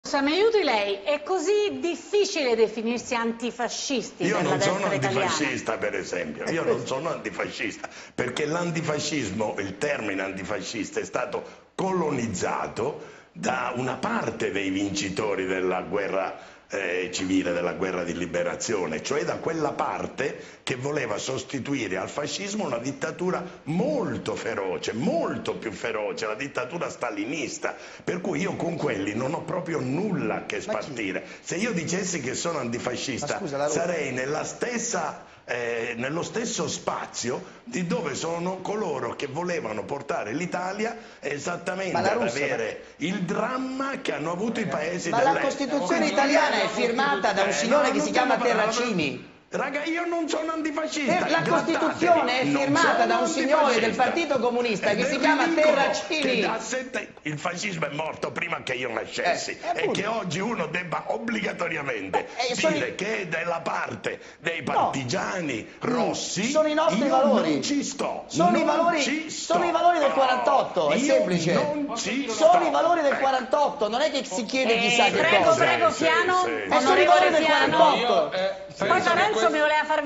Scontro Ignazio La Russa-David Parenzo: "Non sono antifascista", "Sono i valori del '48"
Scontro Ignazio La Russa-David Parenzo: "Non sono antifascista, perché il termine antifascismo è stato colonizzato da quella parte che voleva sostituire il fascismo con lo stalinismo. Con quelli non ho nulla a che spartire" , "La Costituzione italiana è firmata da un signore del PCI che si chiama Umberto Terracini. Sono i valori del '48"